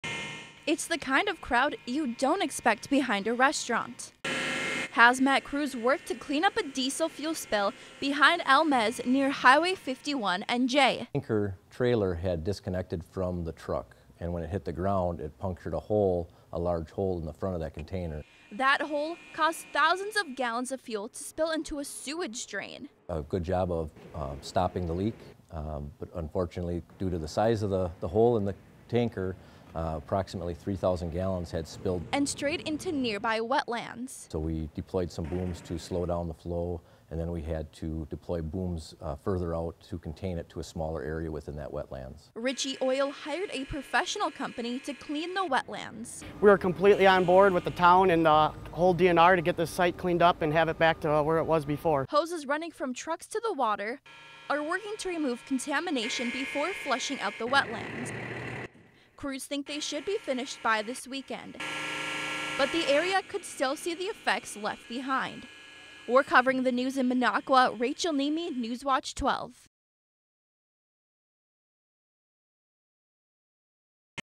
Diesel Spill Clean Up
After a Semi Tanker swerved , the trailer came lose spilling gallons of diesel fuel.
That fuel spilled into nearby wetlands, causing a big cleanup the next day.